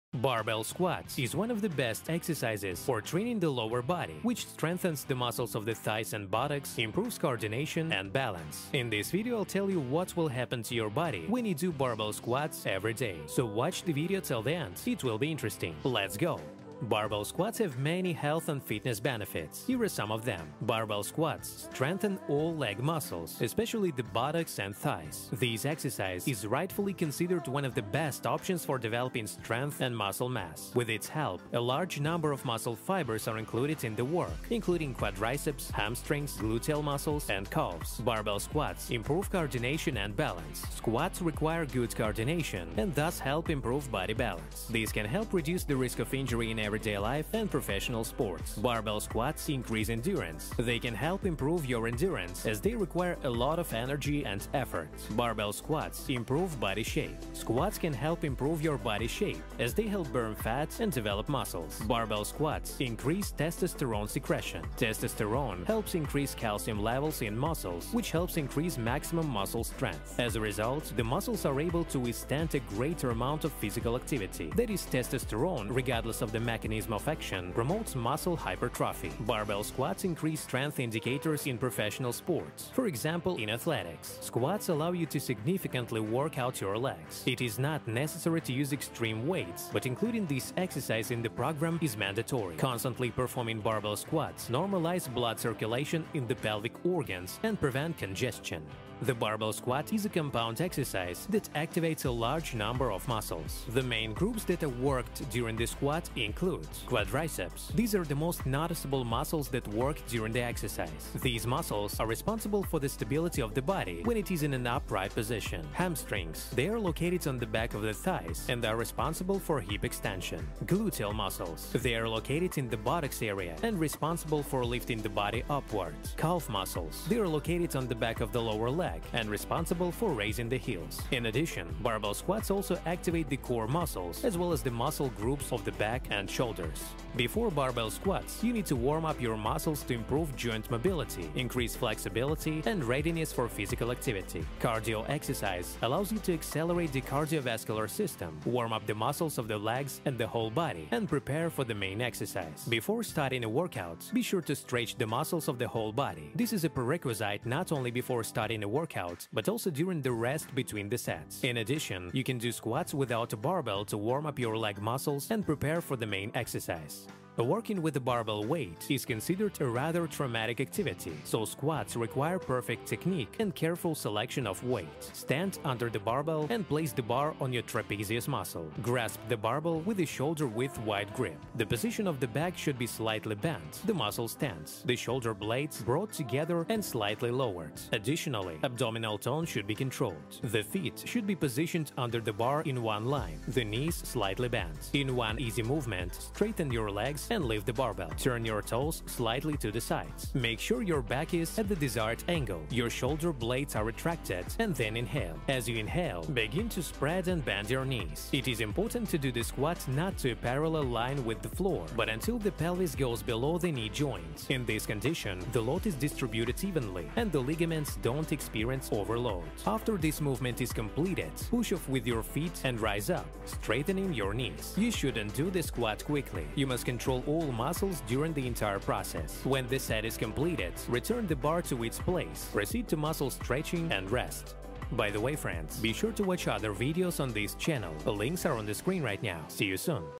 What Happens to Your Body When You Do Barbell Squats Every Day
What Happens to Your Body When You Do Barbell Squats Every Day In this video, I'll tell you what happens to your body when you do barbell squats every day.

00:00 Barbell squats every day
 00:16 Barbell squats benefits
01:38 What muscles do barbell squats work
02:17 How to prepare for barbell squats
02:47 How to do barbell squats properly